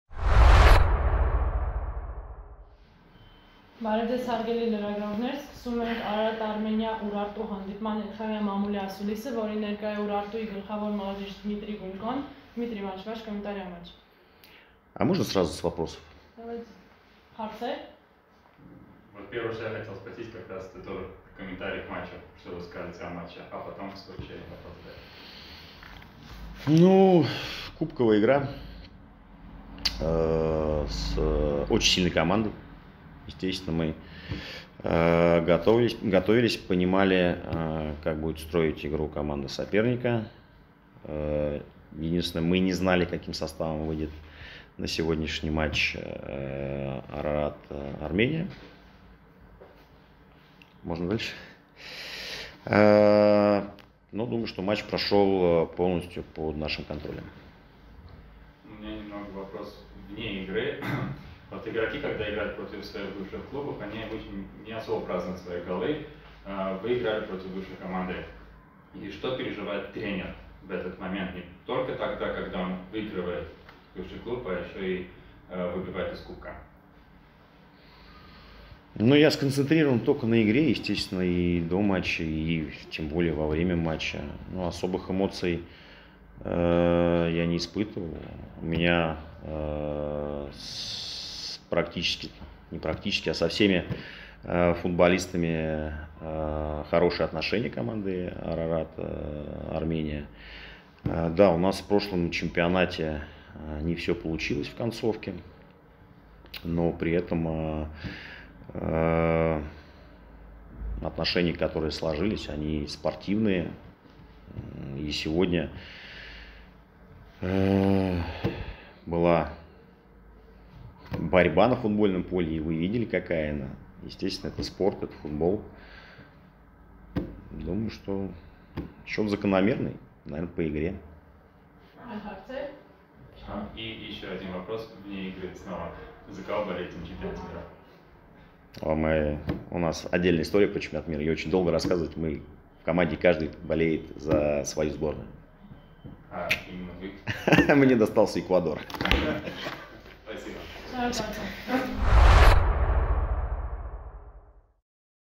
Դմիտրի Գունկոն՝ «Արարատ-Արմենիայի» հետ խաղի մասին | Dmitry Gunko about match with FC Ararat-Armenia
«Արարատ-Արմենիայի» հետ խաղից հետո Դմիտրի Գունկոն հայտնեց խաղի մասին իր կարծիքը․

After the match with FC Ararat-Armenia Dmitry Gunko shared his opinion.